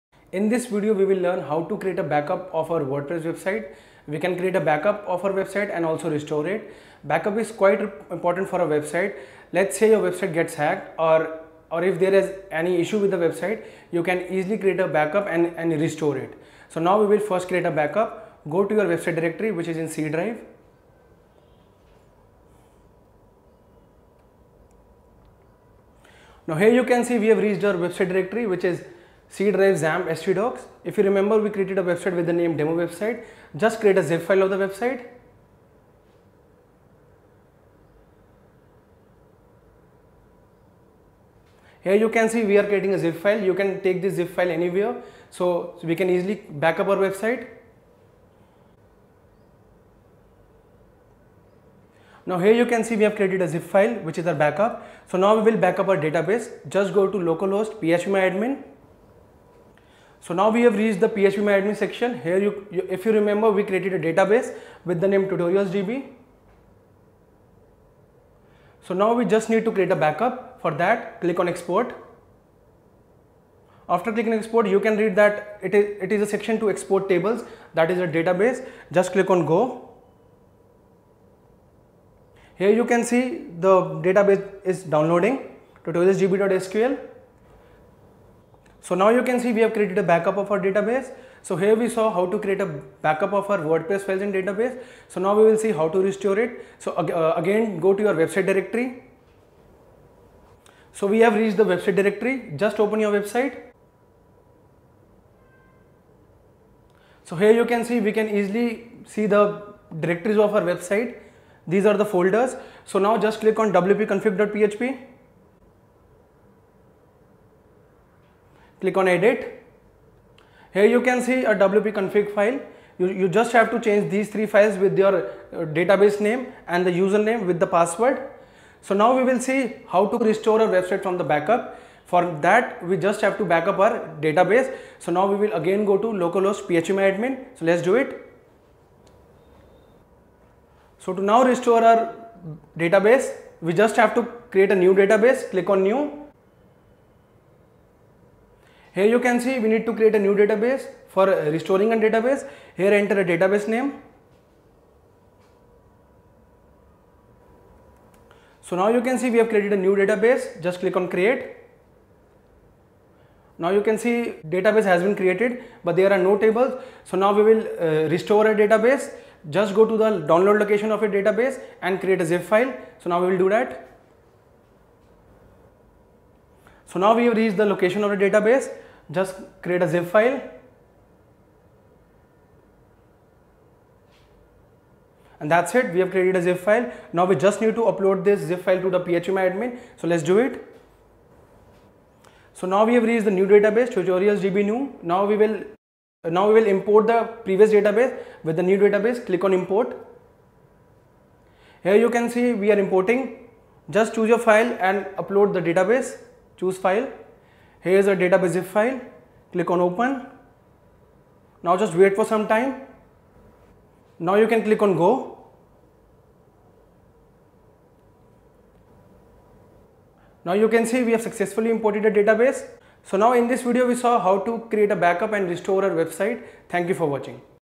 WordPress - Backup & Restore | Tutorialspoint
WordPress - Backup & Restore
Lecture By: Mr. Amit Diwan, Tutorials Point India Private Limited.
In this session, you will learn about Backup and Restore in Wordpress Website.

Wordpress is a Content Management System, which allows the users to build dynamic websites and blogs.